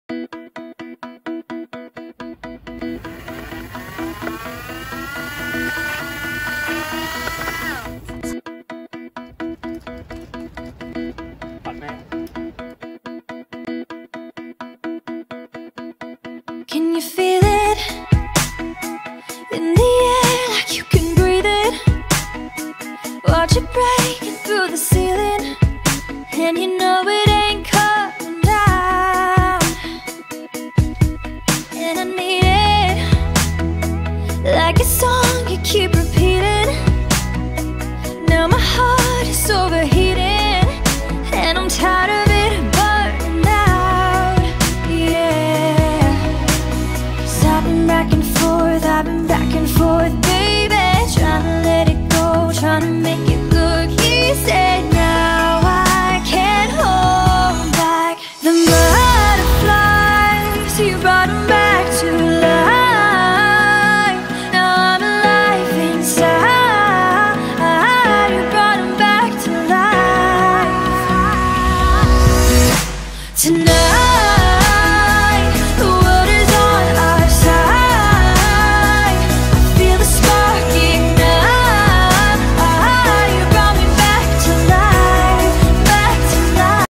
Pretty Cool Zero 10X ! Fantastic shock absorption and acceleration function! You deserve to got it!
Full display of ZERO 10X scooter. Acceleration performance, long stroke shock absorption.